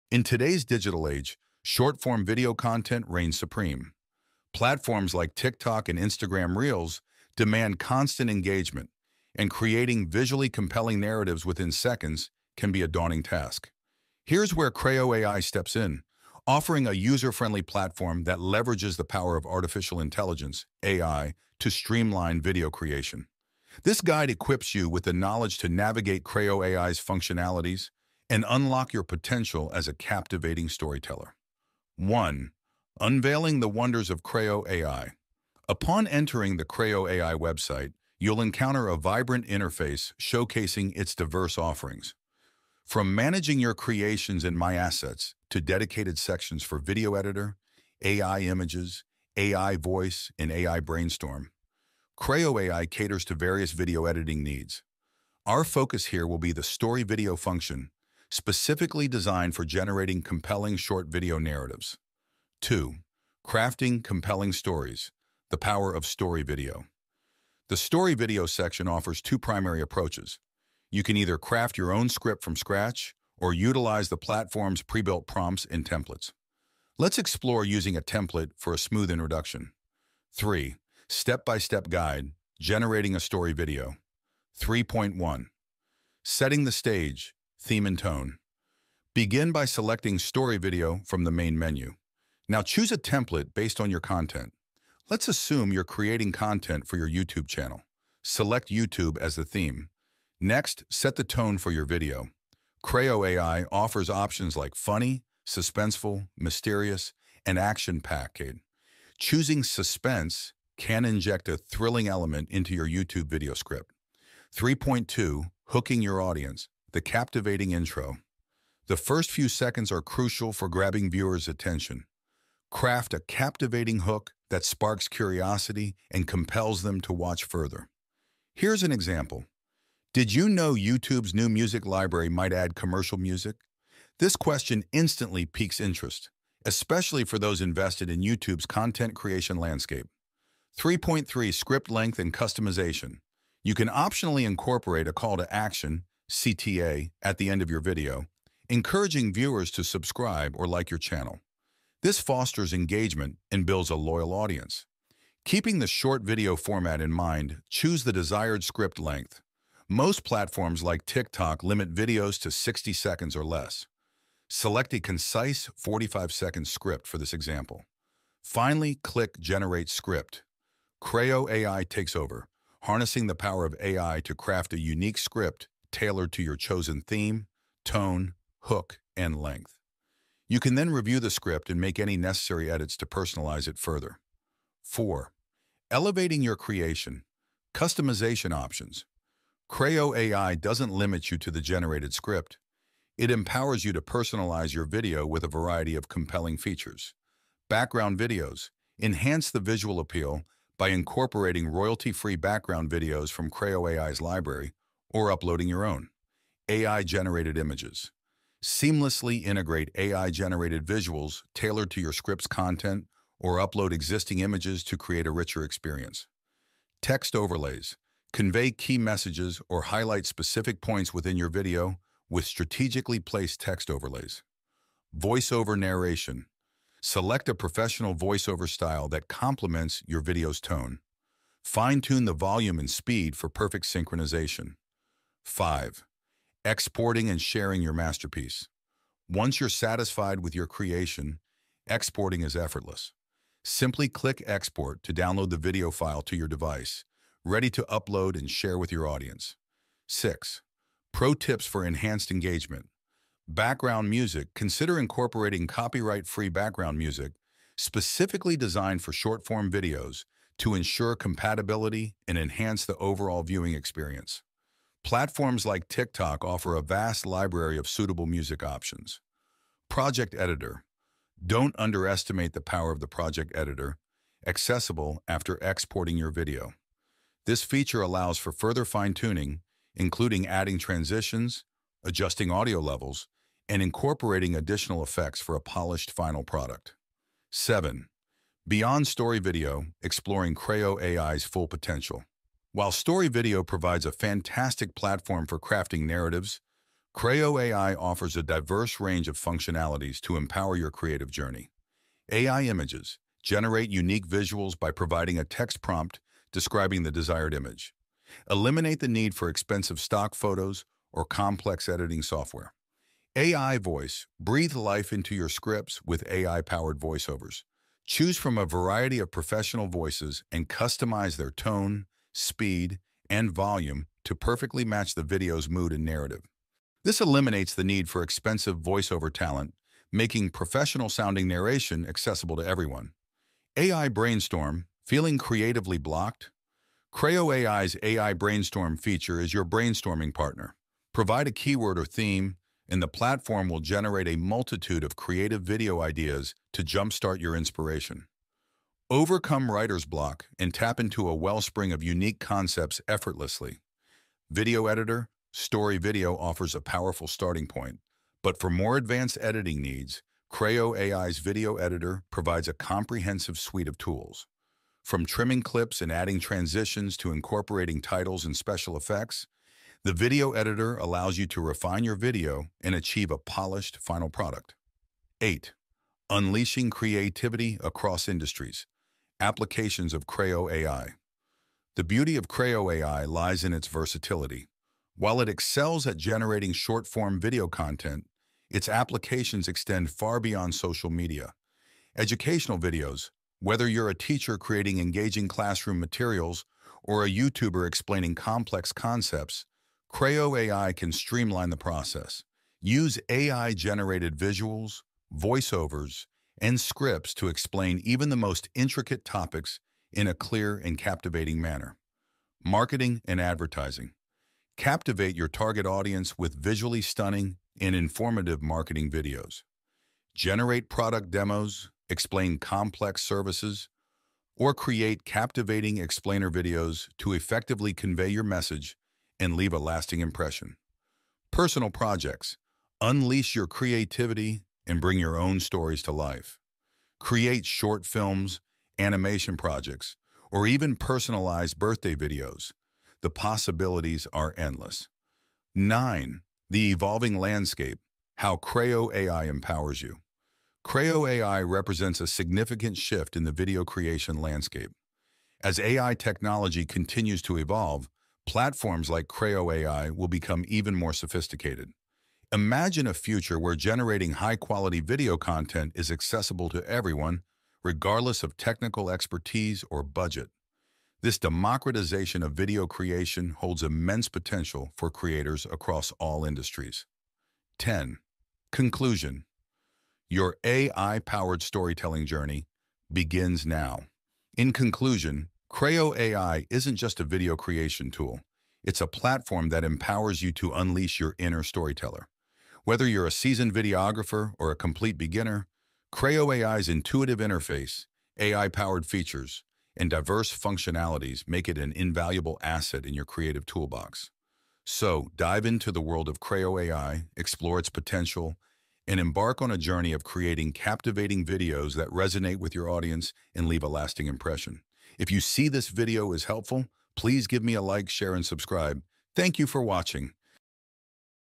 CRAYO AI | Ultimate tool for social media growth !
Are you looking for the ultimate tool to help you grow your social media presence? Look no further than CRAYO AI!

CRAYO AI is an all-in-one social media management tool that can help you with everything from scheduling posts and tracking analytics to creating engaging content and running ads.
These are some AI tools that I often use.
Please, don't copyright !